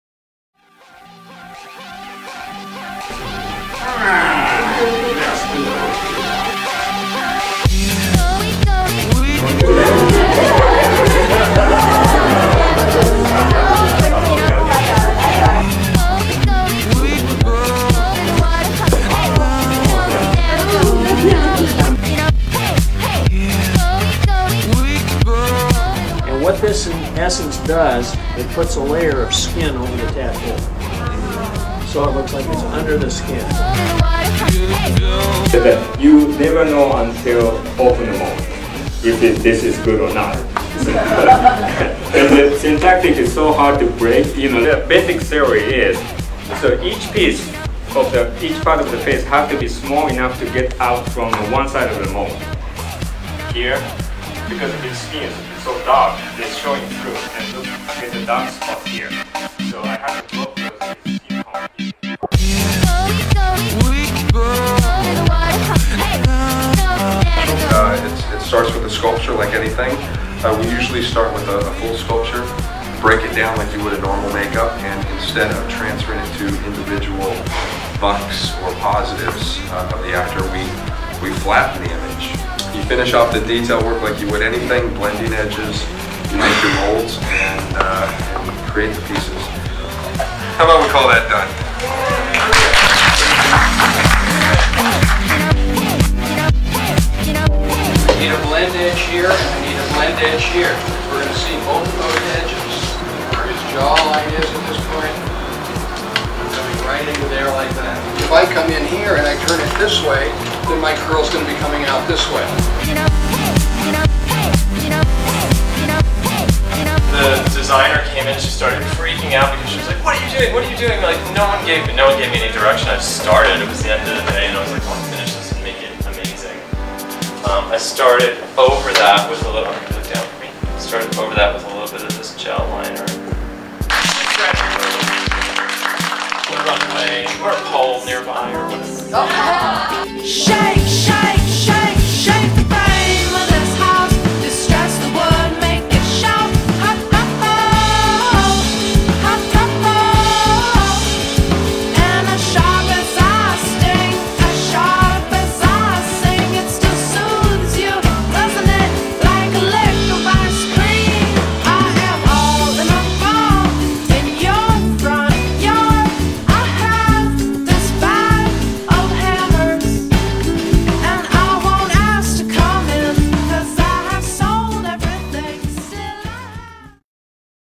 Lecturers at Cinema Makeup School
Lectures, demos, and guest speakers at the Cinema Makeup School.